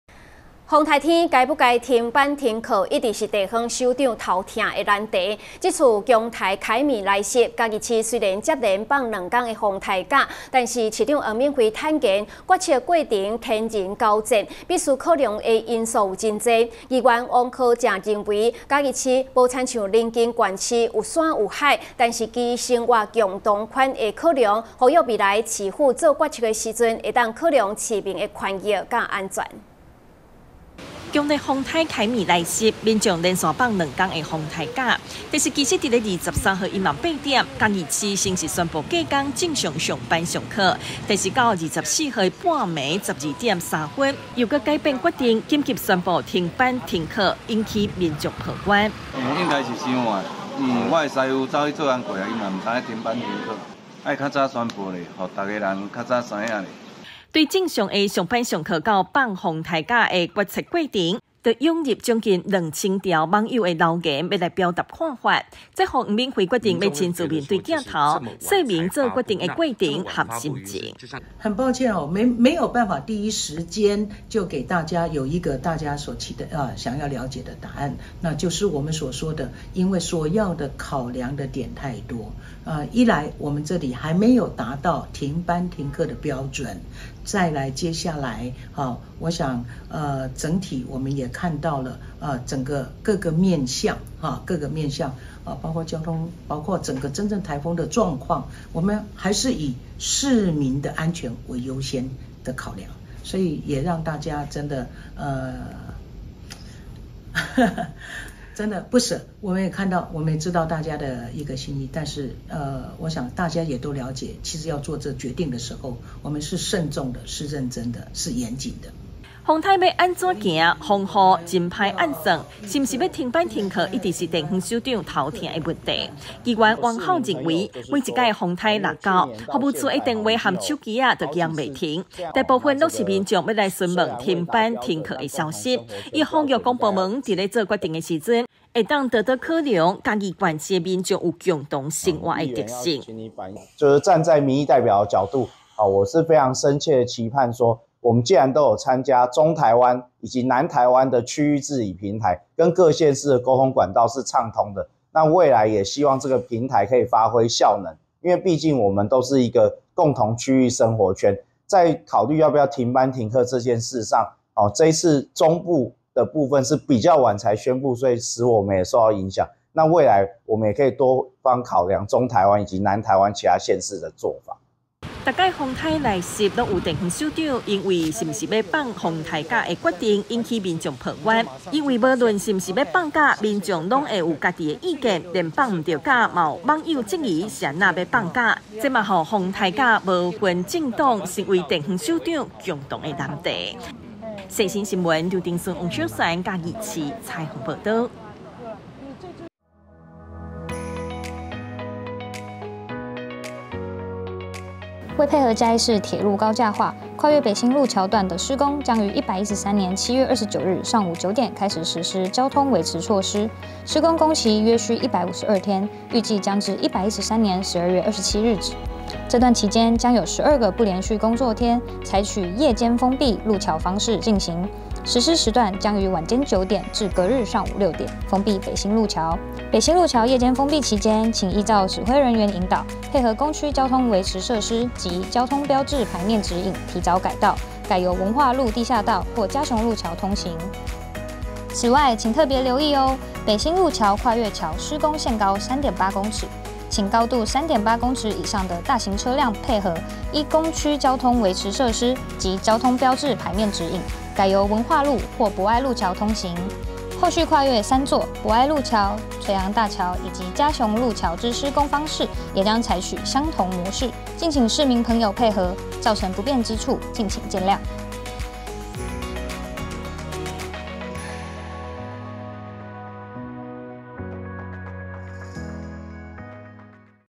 世新新聞 颱風停課班成首長難題 議員籲多考量共同生活圈
113年7月25日 (四) 世新新聞

颱風停課班成首長難題 議員籲多考量共同生活圈

颱風天該不該停班停課，一直是地方首長頭痛的難題，這次強颱凱米來襲，嘉義市雖然接連放了2天颱風假，但是市長黃敏惠坦言，決策過程”天人交戰”，必須考量的因素很多，議員王浩則認為，嘉義市不像臨近縣市有山有海，但基於生活共同圈考量，呼籲未來市府做決策時，能考量市民的權益與安全。